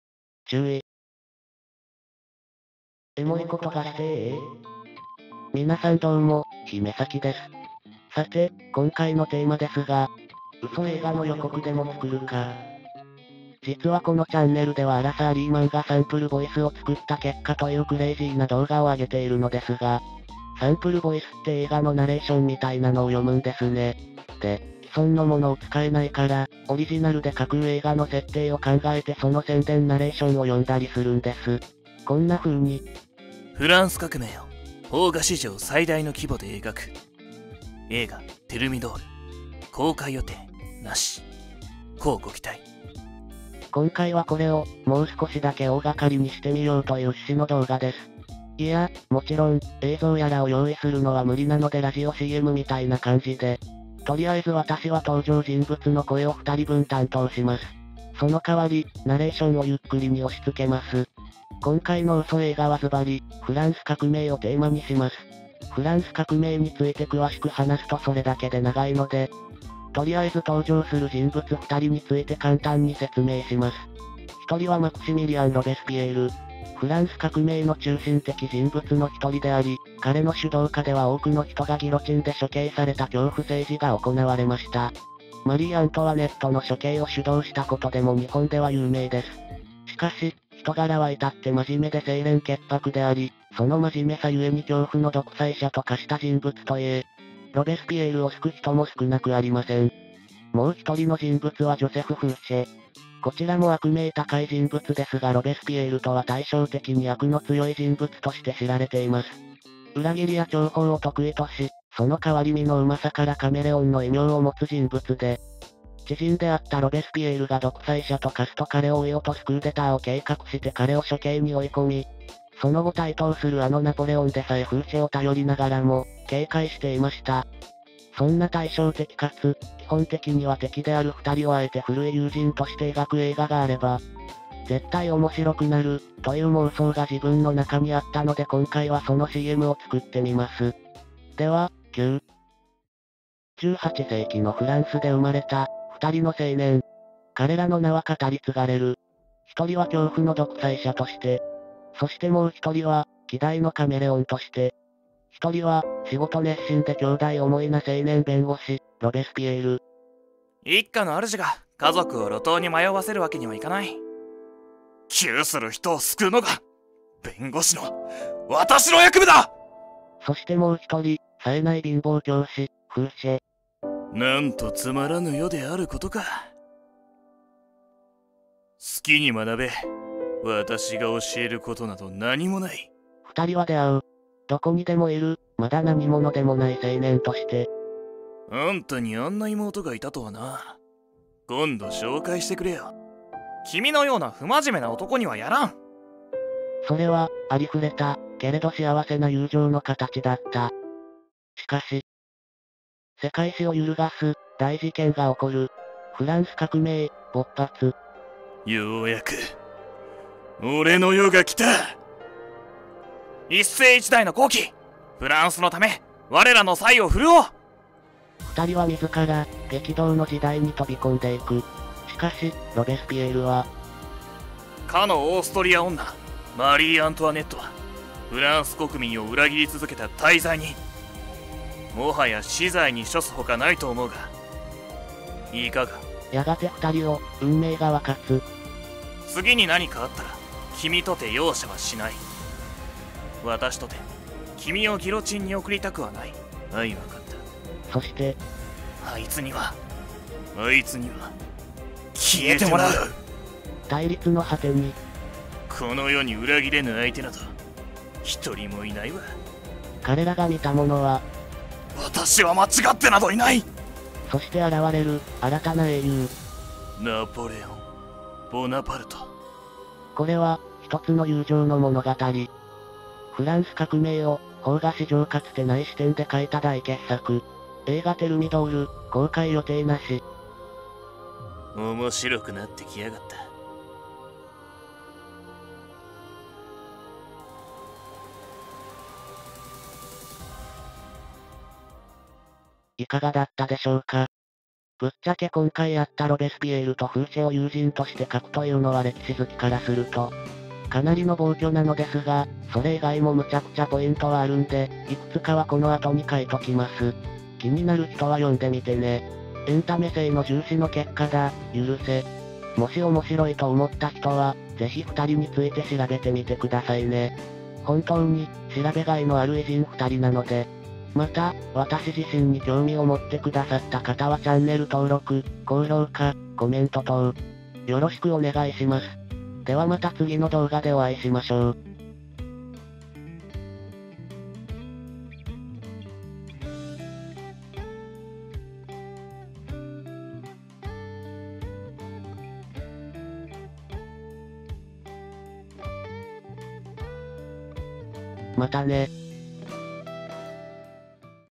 【妄想映画CM】親友・ロベスピエールとフーシェ【ゆっくり×俺】
冴えないサラリーマンのアフレコ、というか演技をお楽しみいただく動画です。このシリーズでは私の『見たい！』が詰まった歴史映画のCM原稿を考え、ゆっくりにナレーションをしてもらい、私が登場人物を演じていきます。今回のテーマはフランス革命。フランス革命期、恐怖政治を主導したマクシミリアン・ロベスピエール、そして知人でありながら彼を追い落として処刑に追い込んだ稀代のくせ者・ジョセフ・フーシェの二人を私が演じます。基本的には対照的で、敵である二人であったらもしその二人が知人どころでなく『ダチ』だったら？
そんな風に描いてくれる映画があれば絶対に面白いのに！
そんな私の妄想をもとに原稿を考えました。IFの世界を描いているため、史実性は皆無ですが、大好きな偉人二人を一生懸命演じましたので、是非この動画で二人の事を知ってほしいです。
【面白くなってきやがった…】（本編より）

お借りしたツール
●ゆっくりムービメーカー４　様
（Aques Talk ライセンス所得済み）
●OtoLogic　様
お借りした画像
●イラストAC　様
●ニコニココモンズ　様
●Wikipedia
●pikaMine 様